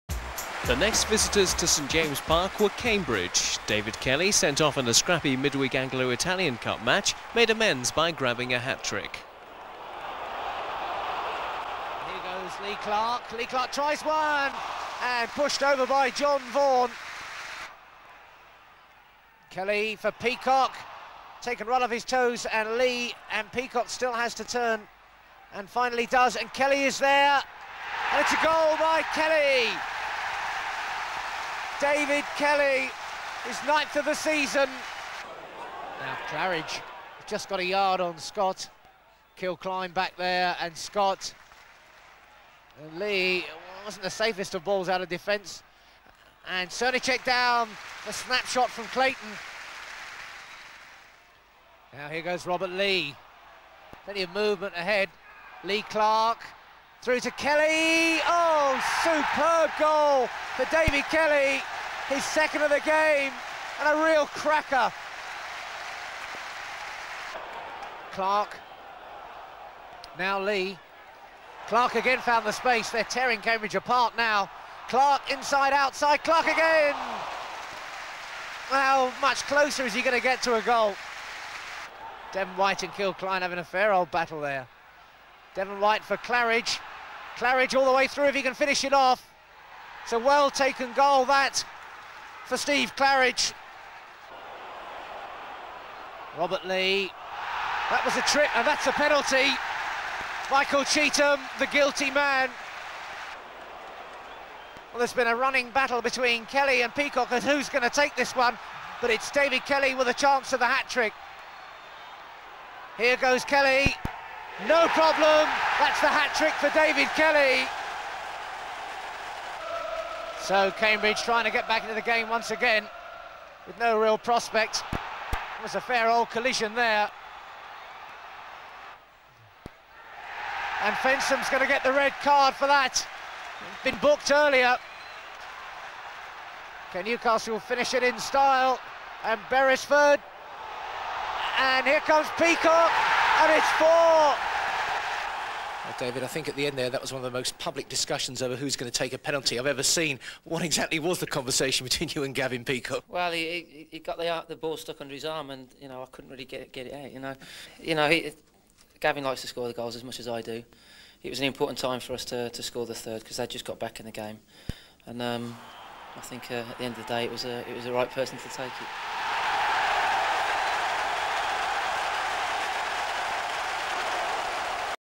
Newcastle United 4 Cambridge 1 - 1992/93 (D1) 28/11/92 - David Kelly hat-trick
Cambridge (19th) - SJP - 27,991
Srnicek, Venison, Beresford, O’Brien, Scott, Kilcline, Lee, Peacock, Kelly, Clark, Thompson
Scorers:  Kelly (3), Peacock - Claridge